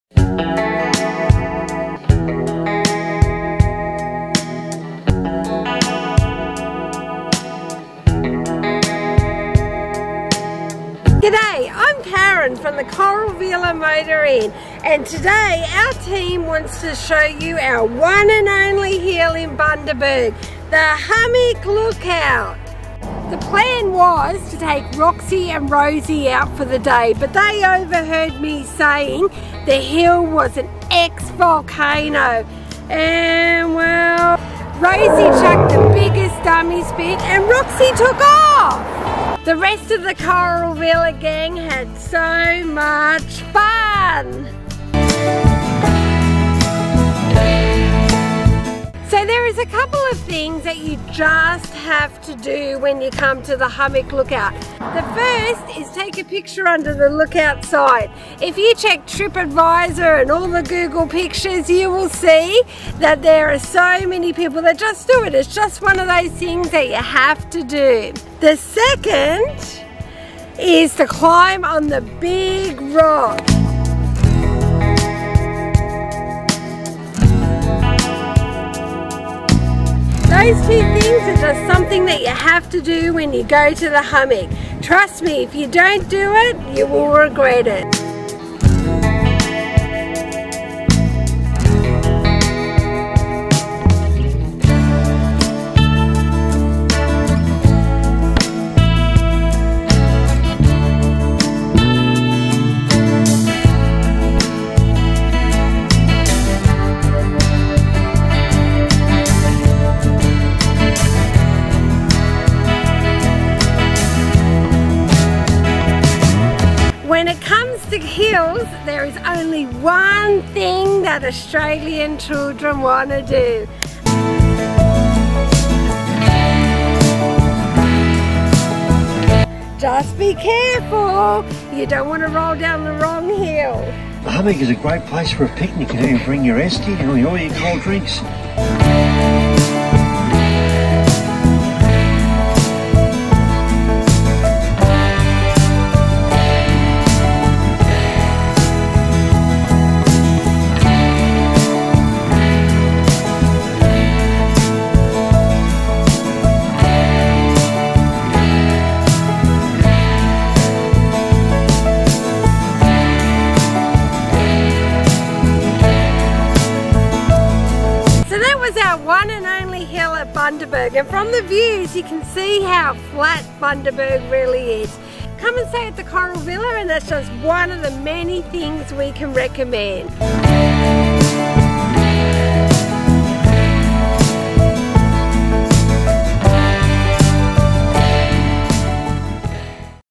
Bundaberg: The Hummock Lookout
The Coral Villa Motor Inn team took a trip out to Bundaberg's one and only hill - The Hummock Lookout - which is an ex volcano - the views are stunning and the team took some AMAZING footage.